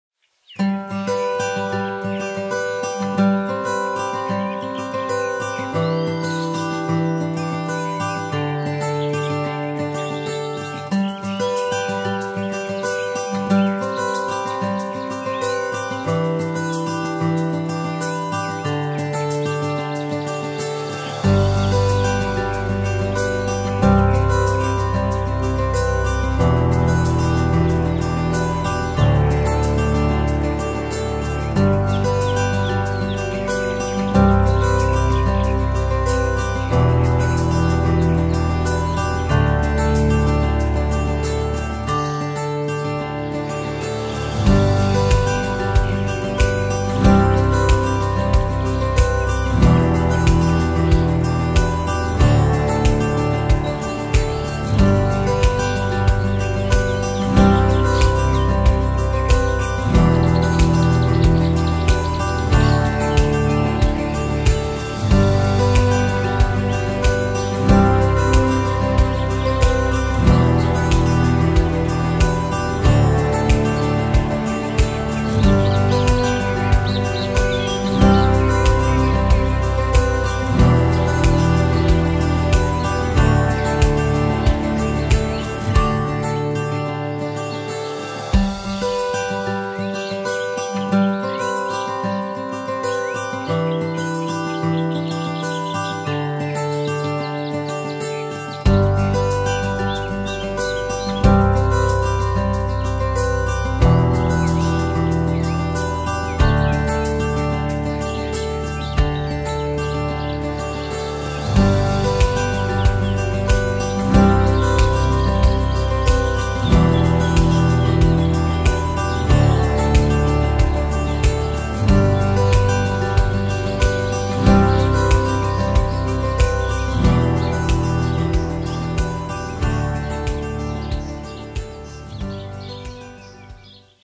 Flamingo and Swan in the middle of Dubai dessert Lake / Filming in C Log with Canon 5d mark iv .
It's quite amazing when you get to see a glimpse of beautiful exotic birds in the middle of a desert especially when you live in Dubai, where it is really rare to see such beauty out in the open.
The best part about this place is that it is absolutely free for the public and the spot is easily found by using a GPS or the road signs itself.